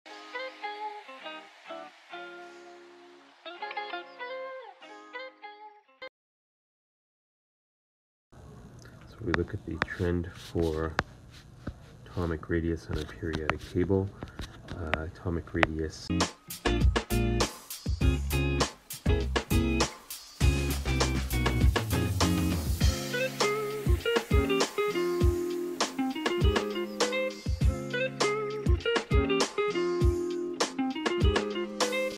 Using only the periodic table, arrange the following elements in order of increasing atomic radius:…
Using only the periodic table, arrange the following elements in order of increasing atomic radius: fluorine, boron, oxygen, neon. Smallest to largest. Answer this question according to the general rules you have learned regarding periodic trends. Do NOT base your answer on tabulated values since exceptions may occur.